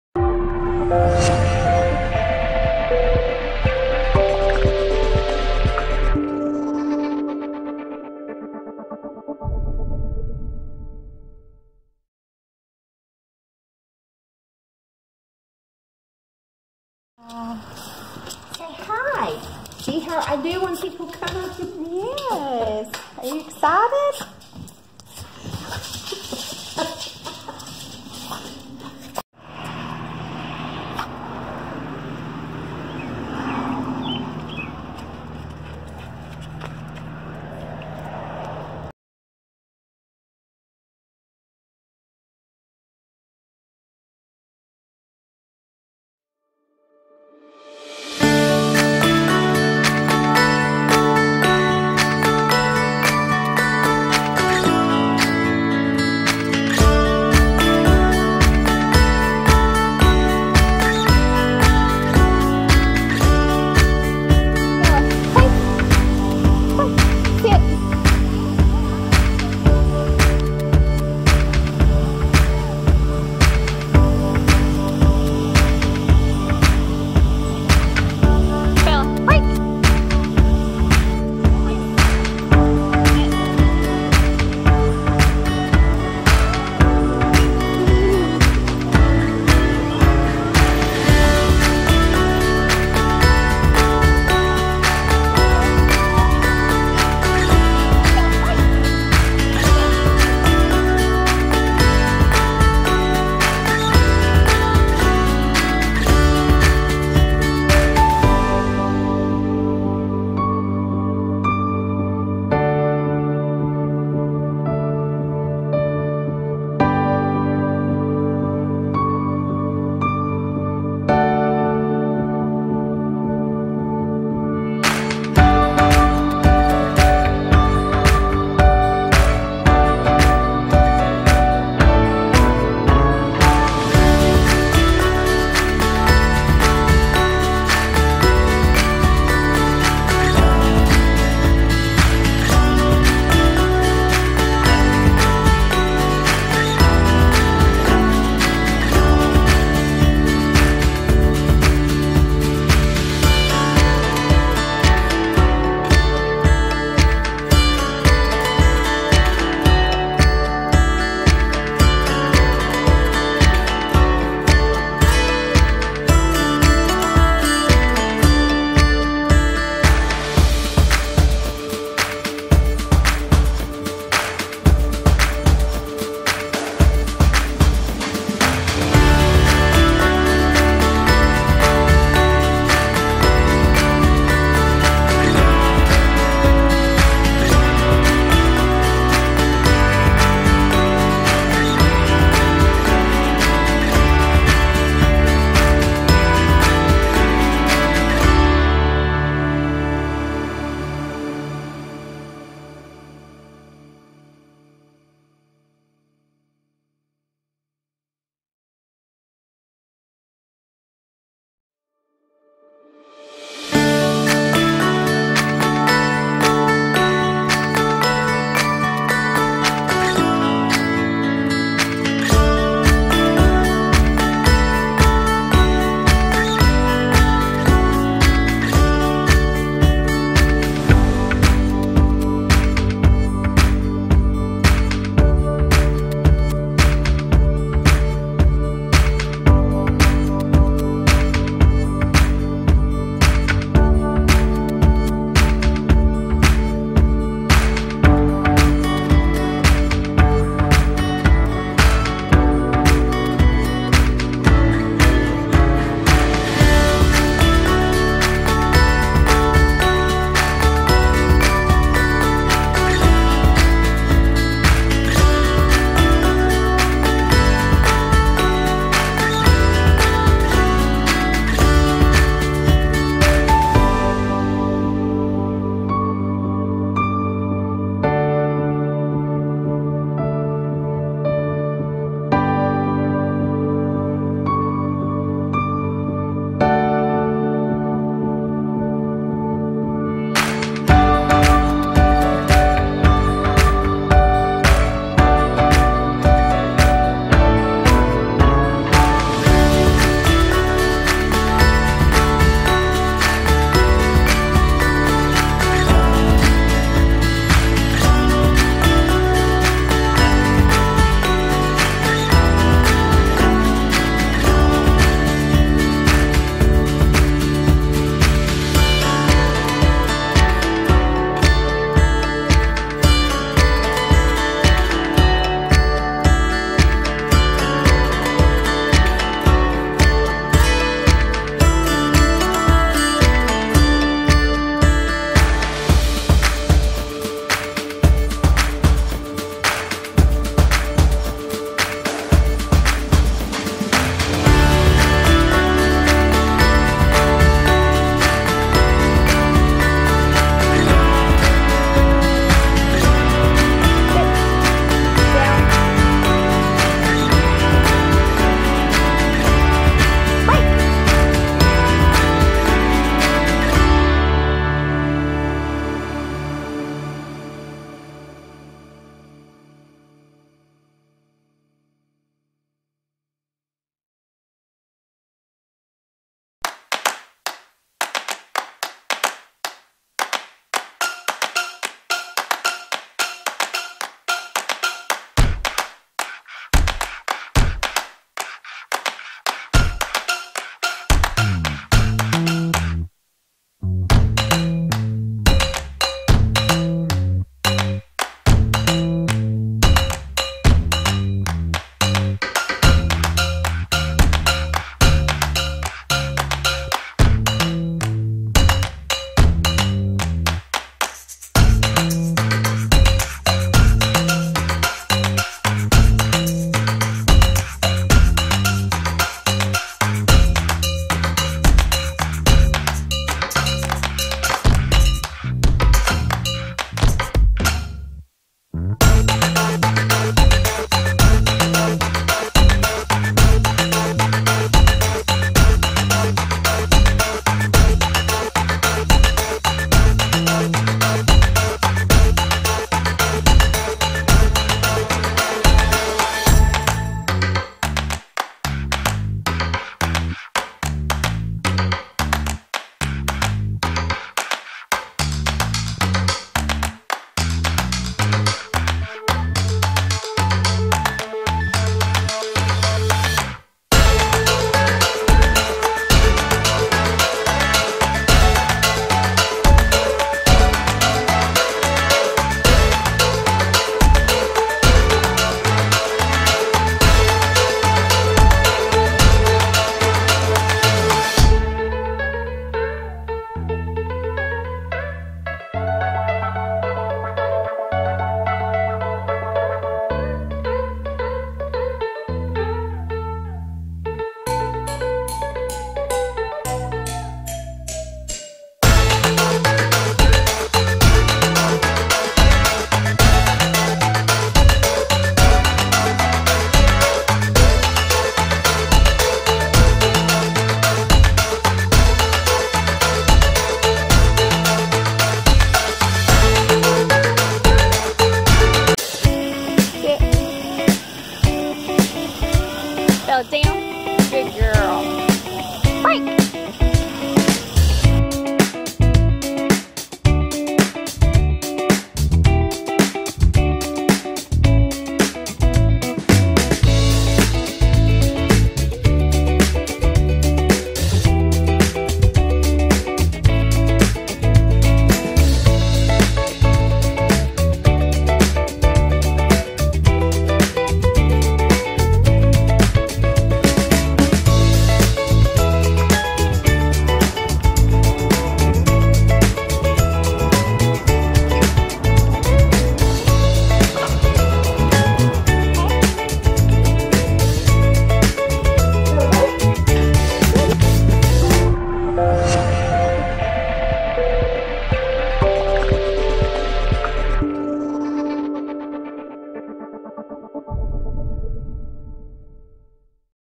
Spaniel learns self control and manners! Off Leash K9 Training TN AL NC
Bella came to us for our 2 week board and train program. Bella's family's biggest complaint was her jumping but states that Bella just didn't have much obedience in general. Check out Bella's progress after just 2 weeks!

Let us help you with your dog!